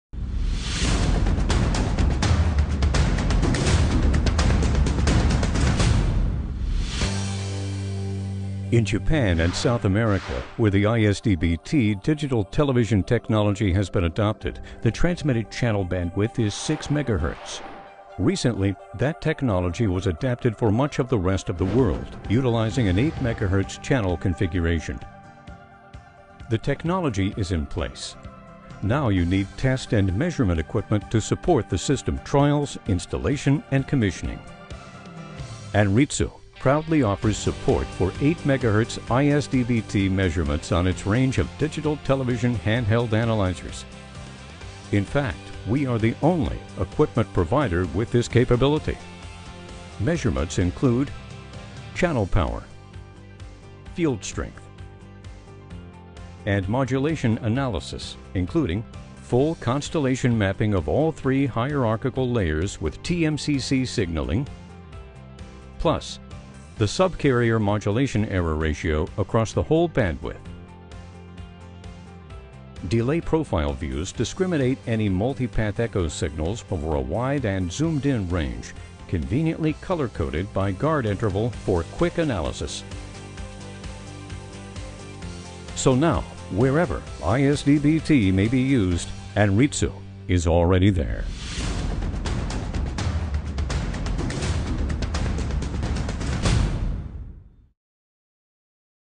Anritsu Supports 8MHz ISDB-T Channel Bandwidths
In Japan and South America, where the ISDB-T digital television technology has been adopted, the transmitted channel bandwidth is 6 Megahertz. Recently, that technology was adapted for much of the rest of the world utilizing an 8 Megahertz channel configuration.

The technology is in place. Now you need test and measurement equipment to support the system trials, installation and commissioning.

Anritsu proudly offers support for 8 Megahertz ISDB-T measurements on its range of digital television handheld analyzers. In fact, we are the only equipment provider with this capability.

Measurements include: Channel Power, Field Strength, and Modulation Analysis including full constellation mapping of all 3 hierarchical layers with TMCC signaling, plus... the sub-carrier modulation error ratio across the whole bandwidth.

Delay profile views discriminate any multi-path echo signals over a wide and zoomed-in range, conveniently color-coded by guard interval for quick analysis.

So now, wherever ISDB-T may be used, Anritsu is already there.